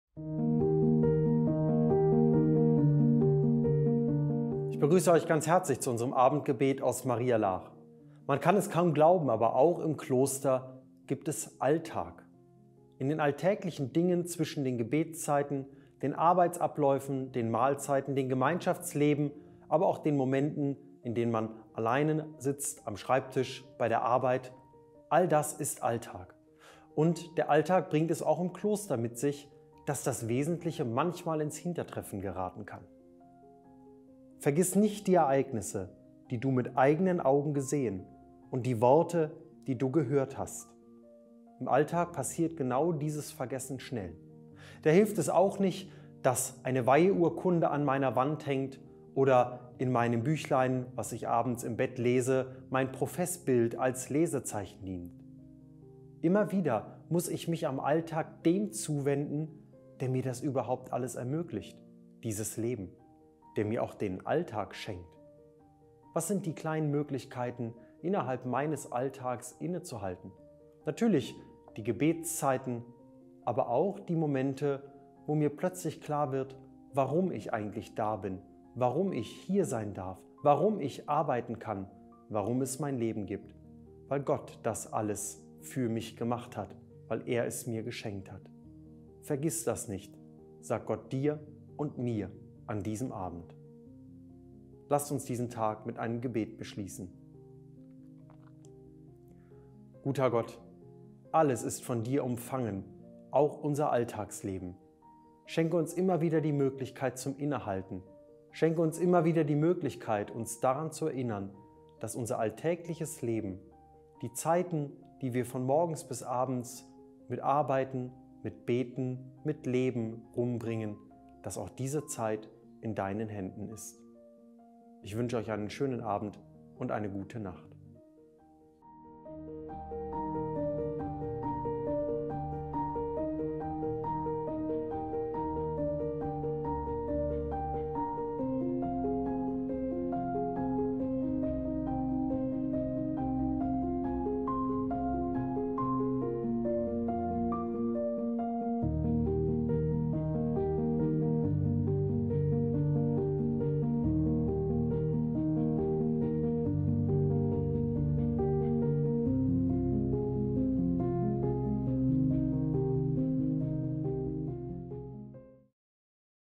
Abendgebet aus Maria Laach 15. Februar 2024
Es ist leicht, im täglichen Leben – selbst im Kloster mit seinen Gebetszeiten, Arbeitsabläufen, Mahlzeiten und dem Gemeinschaftsleben – das Wesentliche zu vergessen (vgl. Dtn 4,9).